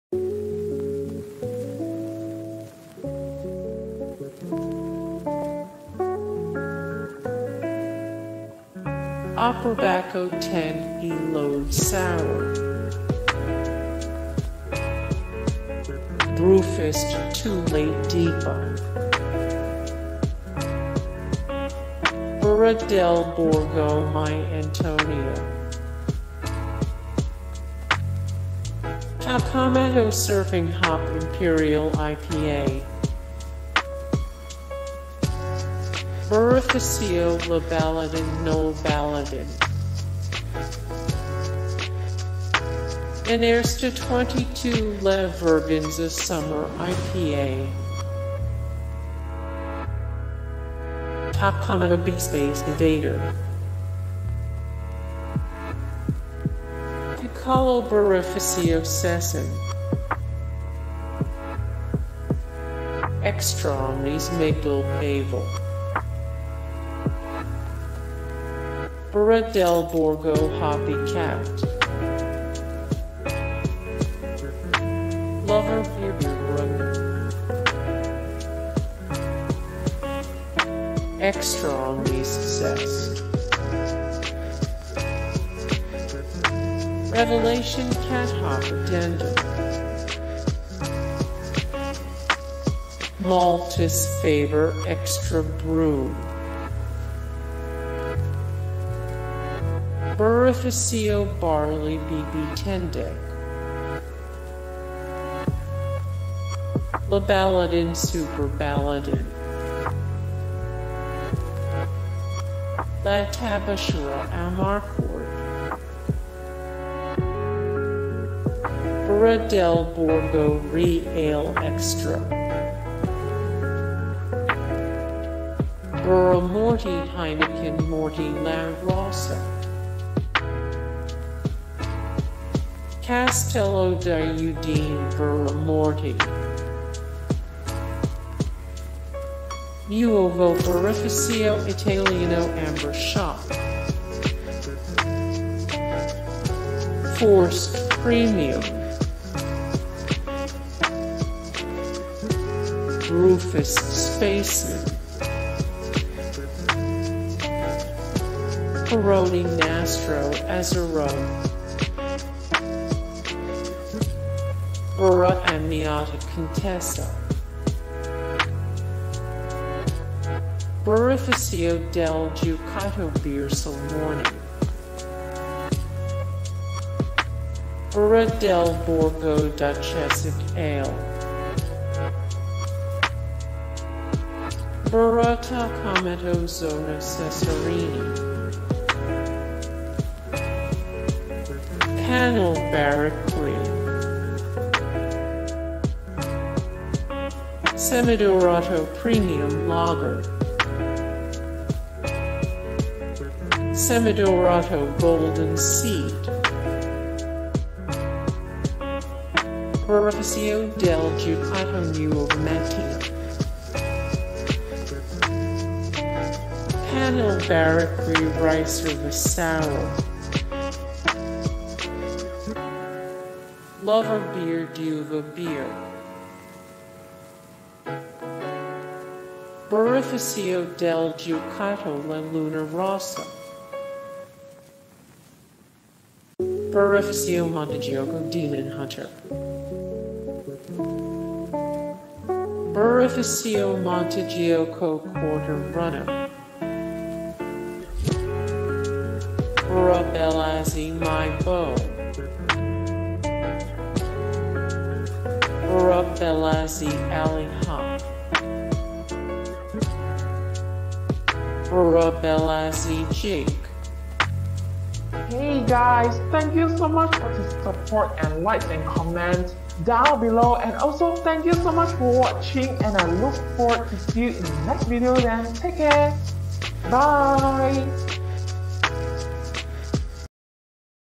The Best Italian Beers 📌
List of top beers brewed in Italy, as chosen by beer enthusiasts, drink experts, and world travelers. Specifically, this greatest beer list incorporates only ales and lagers made and distributed in Italy.Many of these finely crafted drinks originate from the same brewing company, which speak volumes of the fine standards of these companies. Brands like Birra Toccalmatto, Panil, Brewfist, and Birrificio bring artistically designed tastes and presentation to the beer tasting world.This list will assist those who wonder which Italian beers people like the most. Not only native folk, but from people all around the globe will vote on the greatest Italian beers. Co-editors Eugenio Signoroni and Luca Giaccone wrote in 2013'sGuida alle Birre d'Italia (Italian for "Guide to the Beers of Italy") that quality of Italy's breweries is higher than ever with Tuscany alone boasting over 30 microbreweries across its region. Italians normally craft their beers strong and tasty with significant influence from the Belgians leading to beers with high alcohol and flavor content
0:00 - Intro
0:00:08 - Opperbacco 10 E Lode Sour
0:00:16 - Brewfist 2Late DIPA
0:00:23 - Birra Del Borgo My Antonia
0:00:31 - Toccalmatto Surfing Hop Imperial IPA
0:00:39 - Birrificio Le Baladin Nol Baladin
0:00:47 - Menaresta 22 La Verguenza Summer IPA
0:00:56 - Toccalmatto B Space Invader
0:01:03 - Piccolo Birrificio Seson
0:01:10 - Extraomnes Migdal Bavel
0:01:17 - Birra Del Borgo Hoppy Cat
0:01:24 - LoverBeer BeerBrugna
0:01:31 - Extraomnes Zest
0:01:38 - Revelation Cat HopAddendum
0:01:45 - Maltus Faber Extra Brune
0:01:53 - Birrificio Barley BB10
0:02:01 - Le Baladin Super Baladin
0:02:08 - La Tabachera Amarcord
0:02:15 - Birra Del Borgo Re Ale Extra
0:02:23 - Birra Moretti (Heineken) Moretti La Rossa
0:02:31 - Castello Di Udine Birra Moretti
0:02:39 - Nuovo Birrificio Italiano Amber Shock
0:02:47 - Forst Premium
0:02:54 - Brewfist Spaceman
0:03:01 - Peroni Nastro Azzurro
0:03:08 - Birra Amiata Contessa
0:03:15 - Birrificio del Ducato Beersel Morning
0:03:23 - Birra Del Borgo Duchessic Ale
0:03:31 - Birra Toccalmatto Zona Cesarini
0:03:39 - Panil Barrique
0:03:46 - Semedorato Premium Lager
0:03:53 - Semedorato Golden Seed
0:04:00 - Birrificio Del Ducato Nuova Mattina
0:04:08 - Panil Barrique Riserva (Sour)
0:04:16 - LoverBeer D'uva Beer
0:04:23 - Birrificio Del Ducato La Luna Rossa
0:04:31 - Birrificio Montegioco Demon Hunter
0:04:38 - Birrificio Montegioco Quarta Runa
0:04:46 - Birra Bellazzi MyBo
0:04:53 - Birra Bellazzi Alley Hop
0:05:00 - Birra Bellazzi Jake